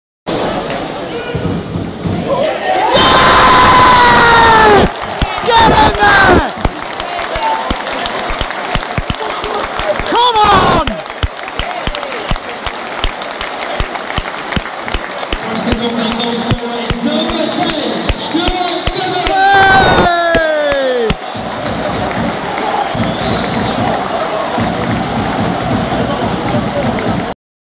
Stuart Beavon Vs Oldham
Stuart Beavon deflects Joel Byrom's free-kick into the net to put Preston 1-0 up against Oldham (apologies for my reaction!)